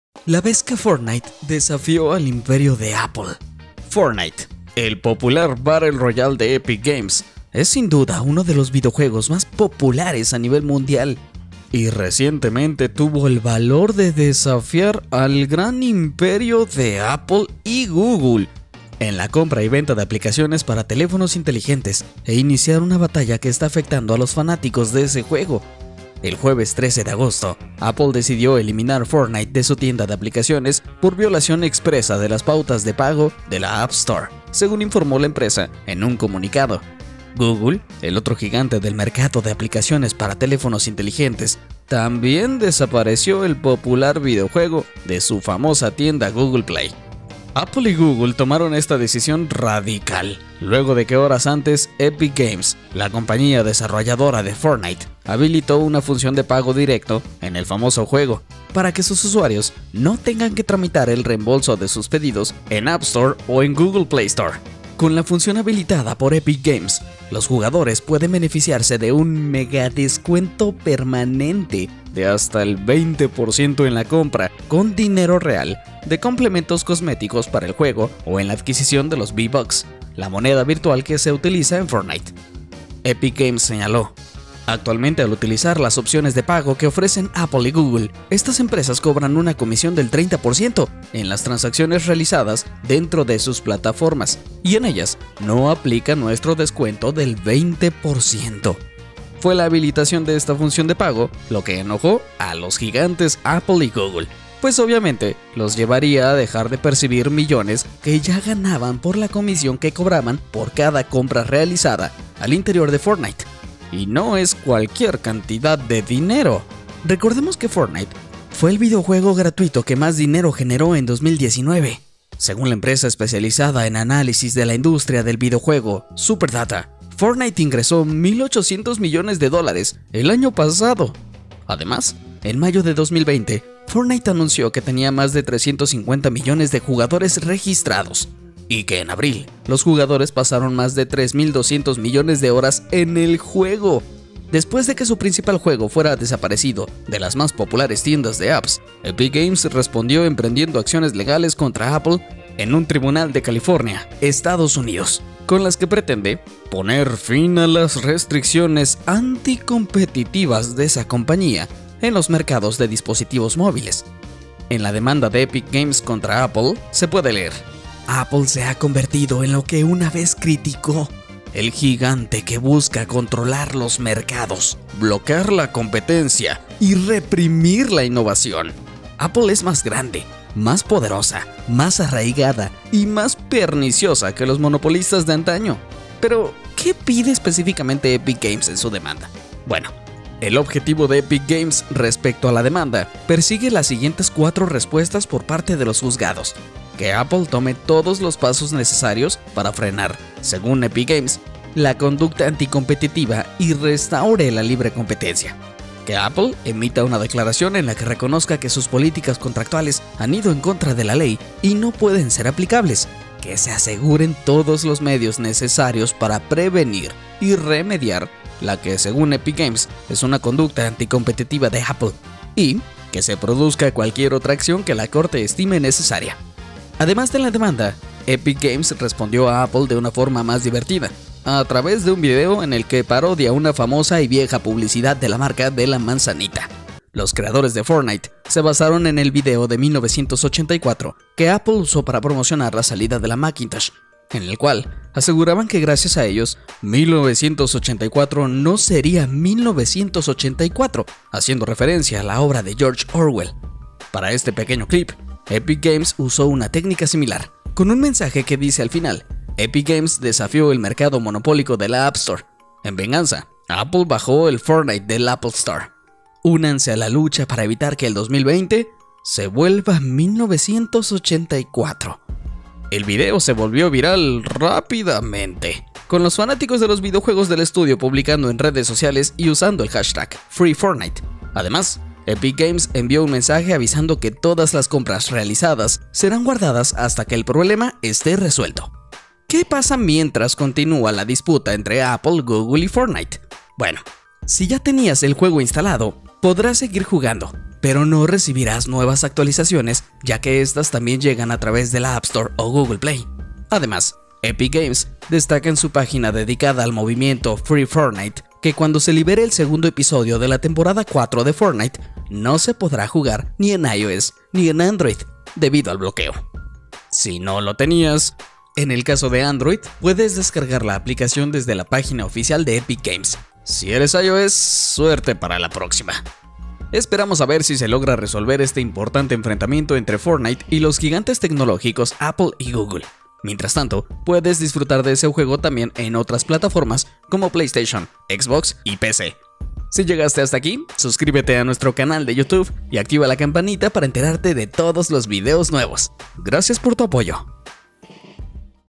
La vez que Fortnite desafió al imperio de Apple
Fortnite, el popular Battle Royale de Epic Games, es sin duda uno de los videojuegos más populares a nivel mundial y recientemente tuvo el valor de desafiar al gran imperio de Apple y Google en la venta y compra de aplicaciones para teléfonos inteligentes e iniciar una batalla que está afectando a los fanático de ese juego.